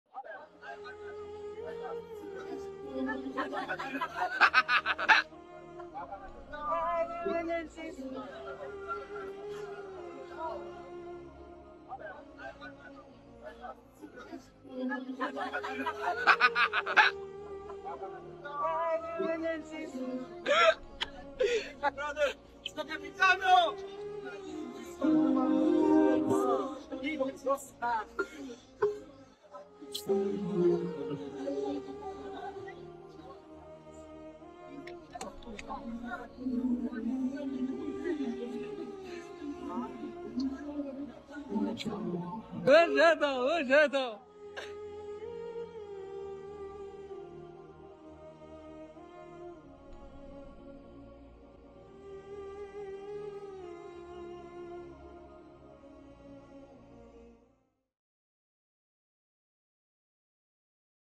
EMOTIONAL: Mother Surprises Son at World Cup
A mother surprised her son who was working as part of the security team at the Qatar